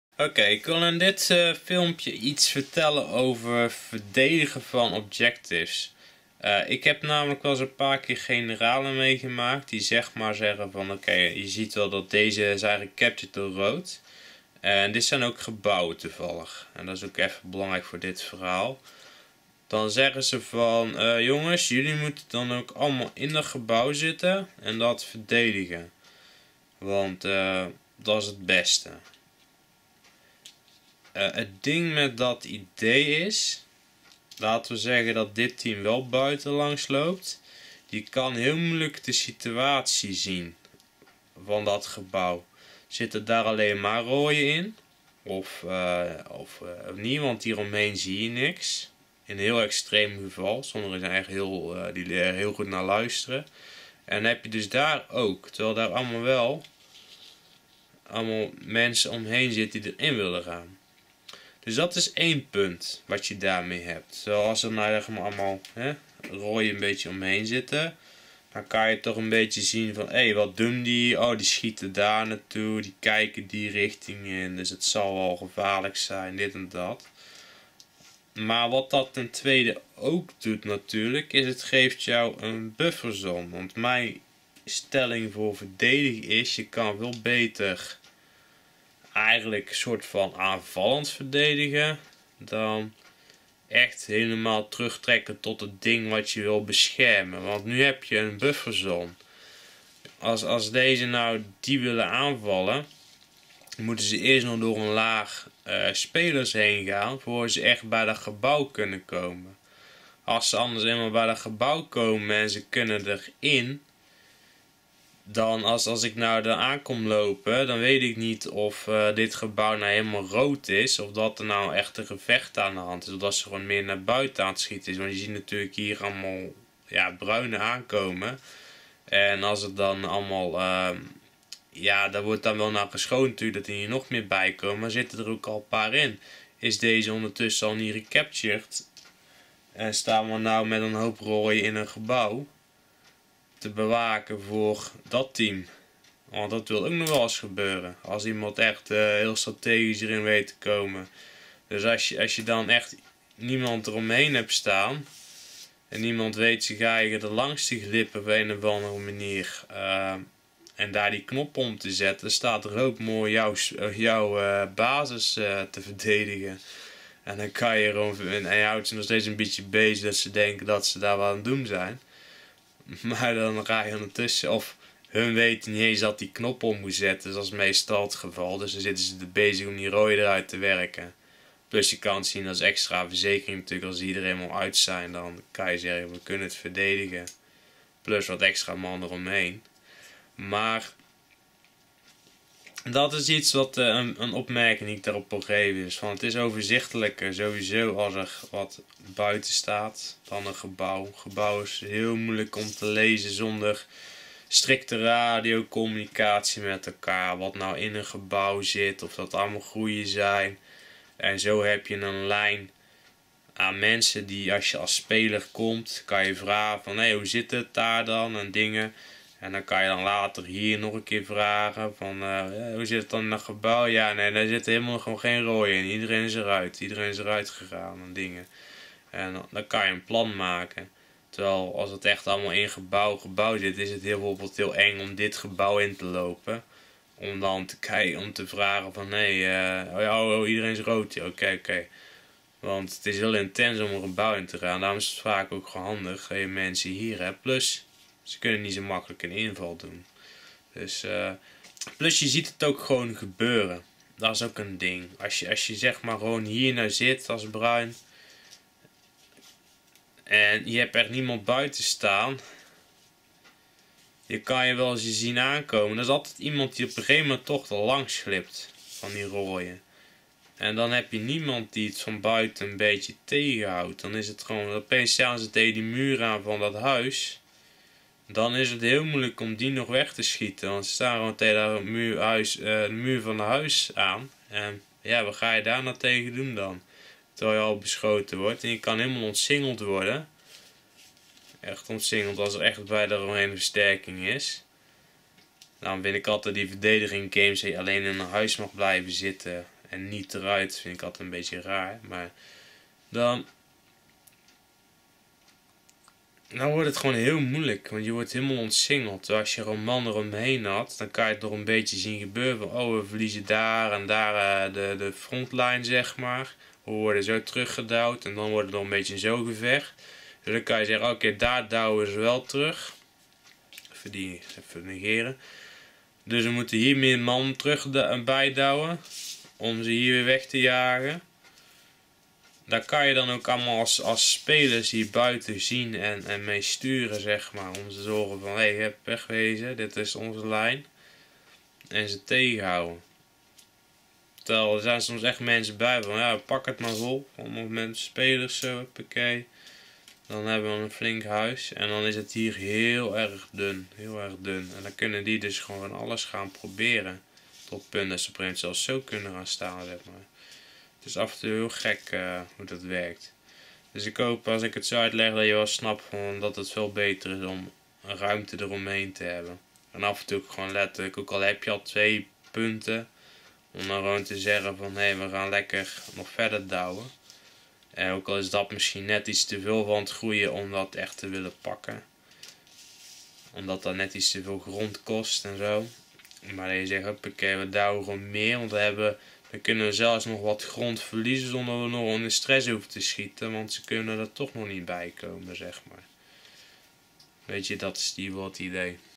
BEERen TIPS: Veld Verdeling - objectives verdedigen
hier geef ik wat tips over het verdedigen van objectives tijdens een paintball game

je kan hier de andere filmpjes zien.